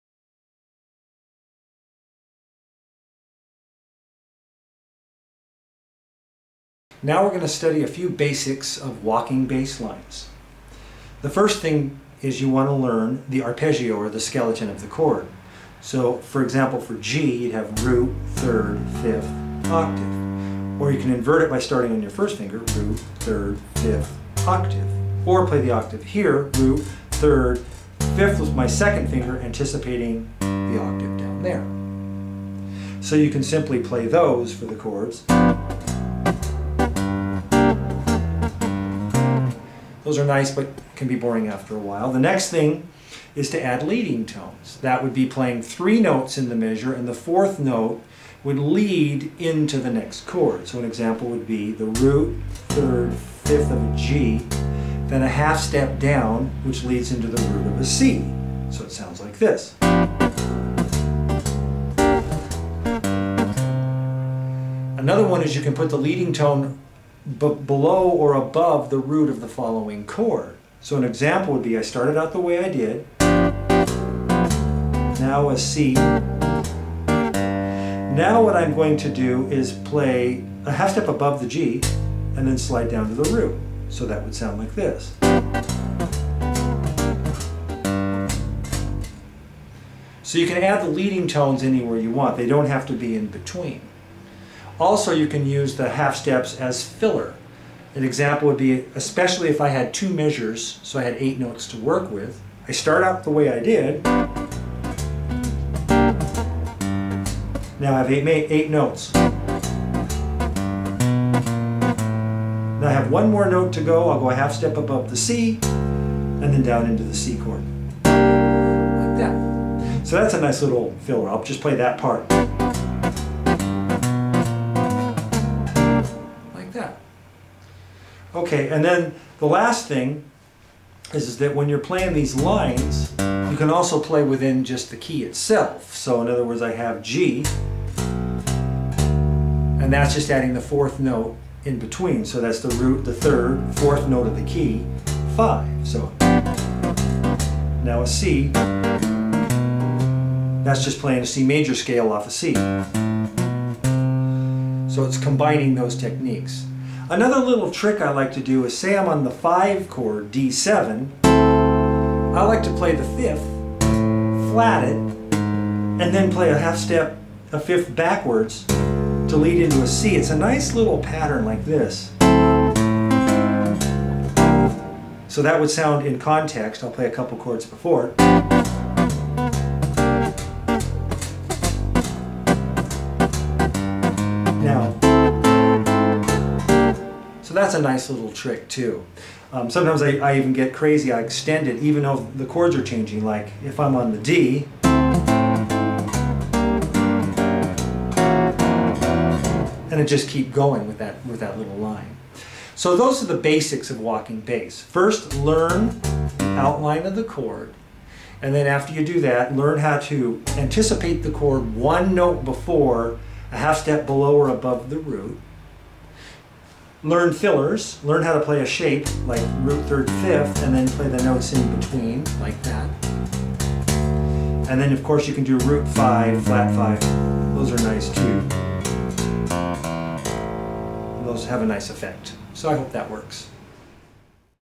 Chapman Stick lesson, Beginning Walking Bass
This lesson works with beginning walking bass line construction. Beginning-Intermediate